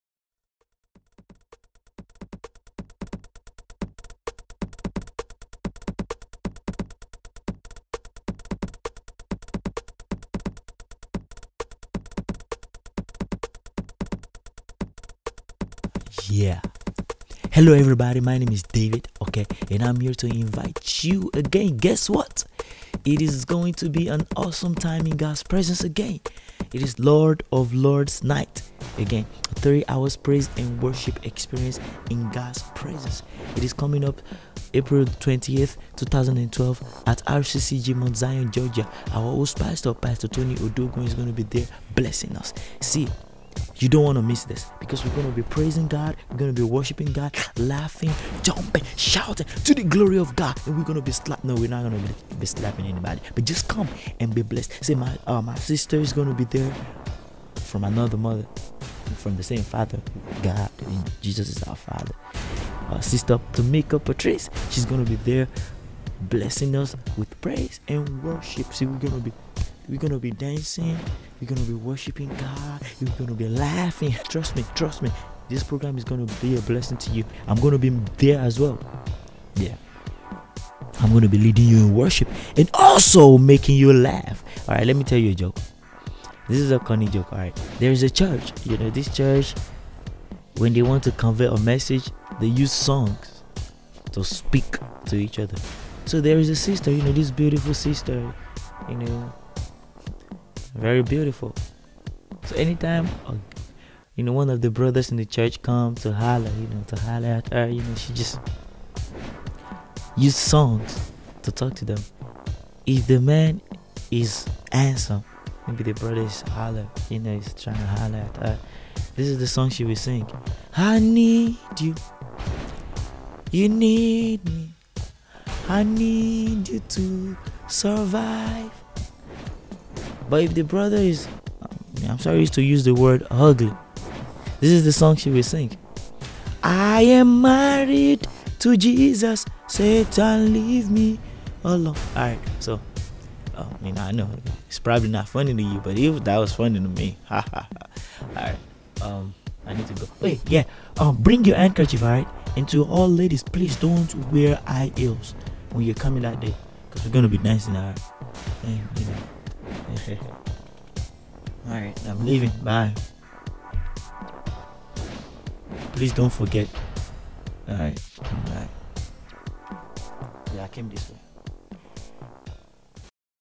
LORD of lords Night AD APRIL 28TH 2012 Part 2
R.C.C.G. Mount Zion Parish Ga
in Conjuction with
AGS MUSIC MINISTRIES

invites you to LORD of lords night

YAY! It's here again the quarterly (every 4month) 3 Hours Praise & Worship Experience In His Presence. Bringing people of all races, culture, background and every part of the world to worship God.

Pastor Tony Odugunwa
(HOST PASTOR)

Featuring:
Restoration Voices
Voices of Praise
Tameka Patrice
Pastor & Pastor (Mrs.) Ebenezer & Olu Ikenebomeh
David Oke (AGS a.k.a McRhema)
Mount Zion Youth Choir & Lol Crew